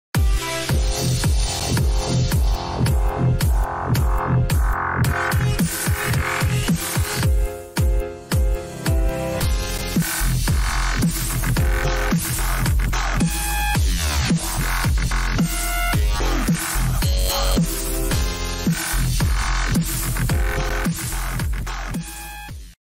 [Gameplay] My part in "???" hosted by luke06
Good rukkus song, fun building even though I kept deleting and rebuilding cause I was never happy with the result. I like how it ended up though.